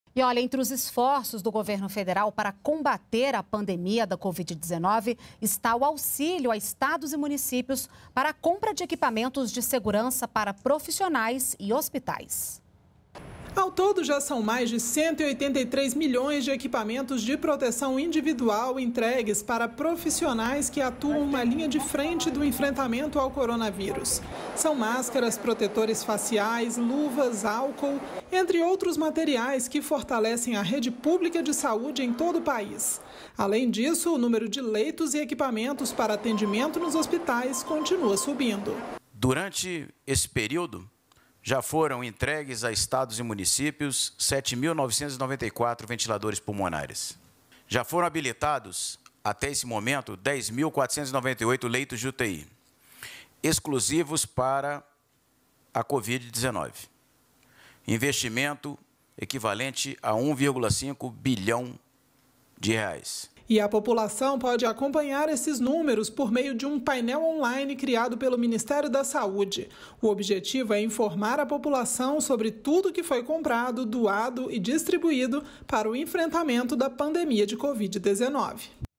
Estados e municípios receberam mais de 180 milhões de EPIs para combate à Covid-19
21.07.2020 - Governo Federal já entregou 183 milhões de equipamentos de proteção individual a estados e municípios para combate ao novo coronavírus.